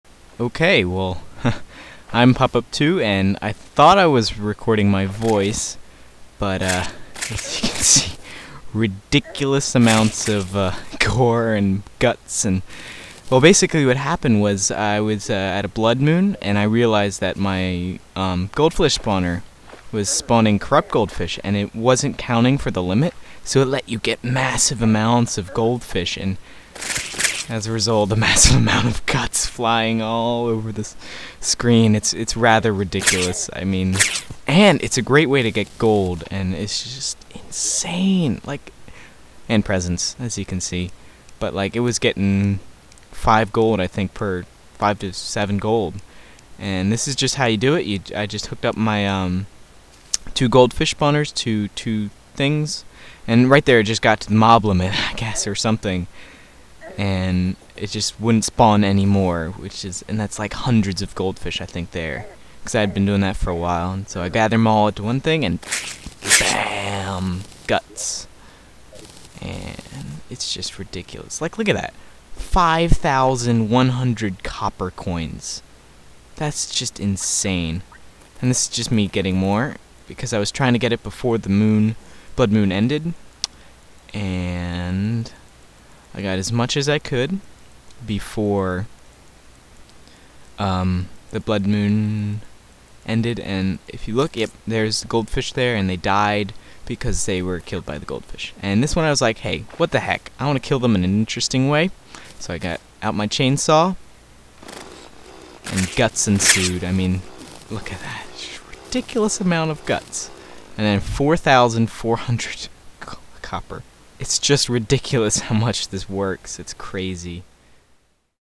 Goldfish Lulz
I found something fun to do during Blood Moons.

Also, can you tell me if you like the ovelay, it was fun to make but I was just wondering if you guys liked it.